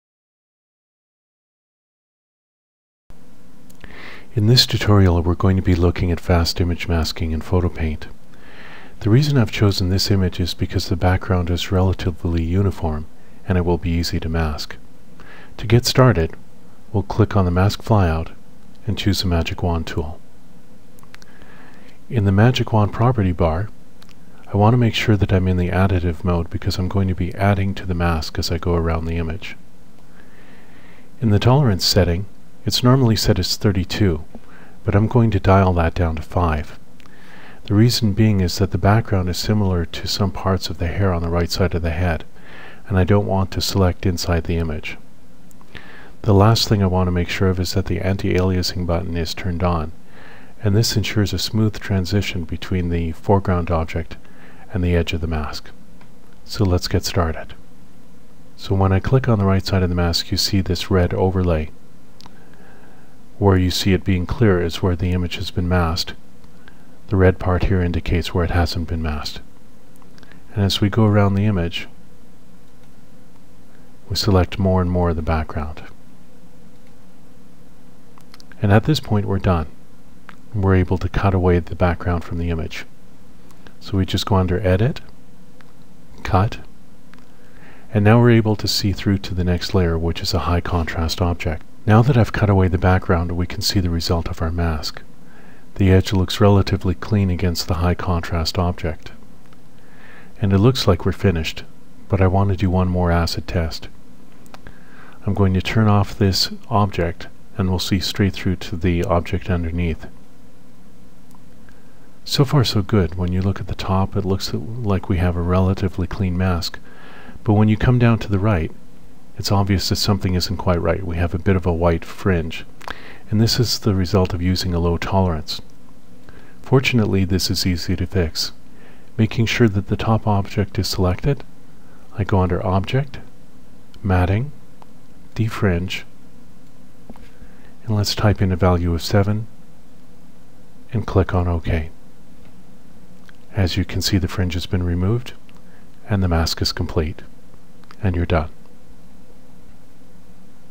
Fast Masking in Corel PHOTO-PAINT X3
Here's where you learn fast image masking in Corel PHOTO-PAINT X3. You'll learn the basics of masking using the magic wand tool, what to do to simplify the process and how to use the finished mask in your composites/compositions.